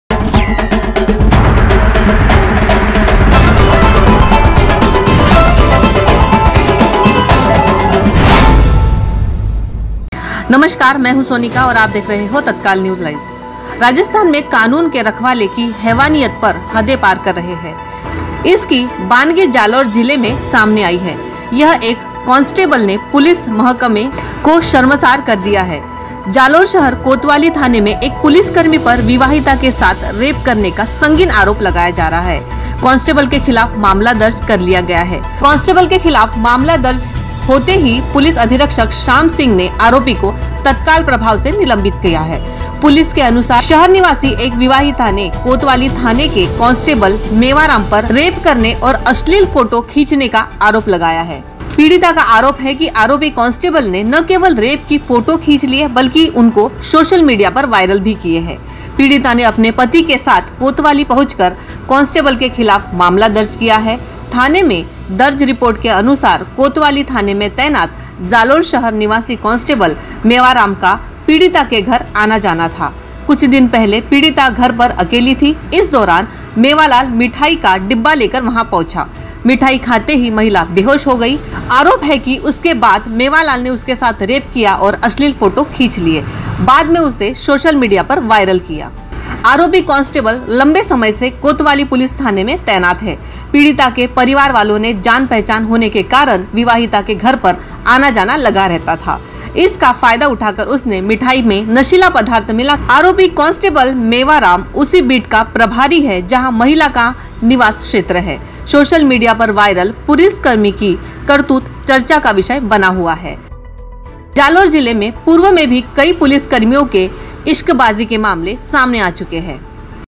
राजस्थान पुलिसकर्मी ने किहैवानियत की हद पार || विवाहिता के साथ रेप फिर || TatkalNewslive
जरस्थान पुलिसकर्मी ने किहैवानियत की हद पार || विवाहिता के साथ रेप फिर